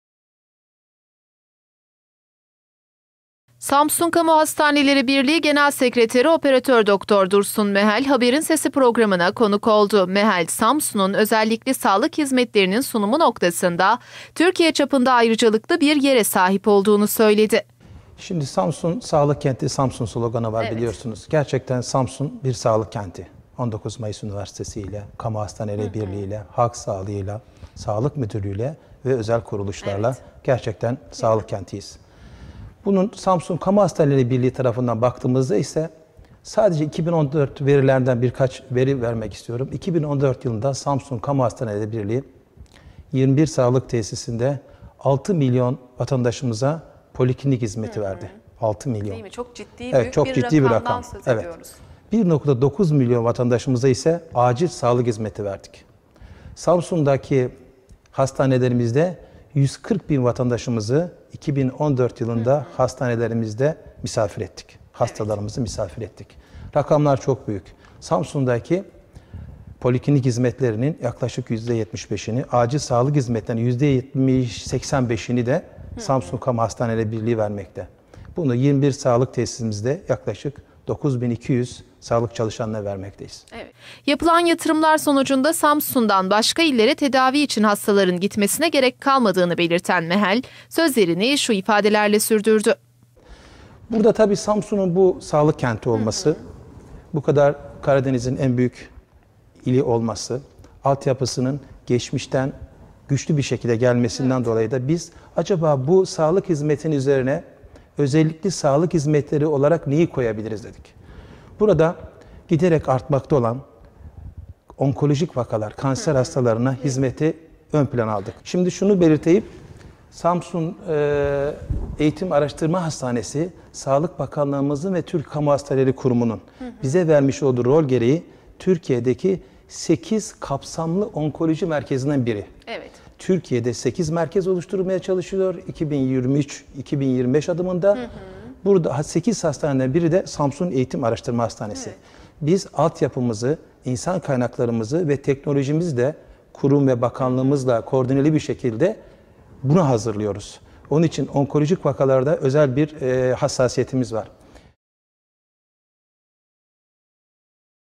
SAMSUN SAĞLIK KENTİ
Samsun Kamu Hastaneleri Birliği Genel Sekreteri Op. Dr. Dursun Mehel, yapılan çalışmalarla Samsun'un tam anlamıyla bir sağlık kenti halbine geldiğini söyledi.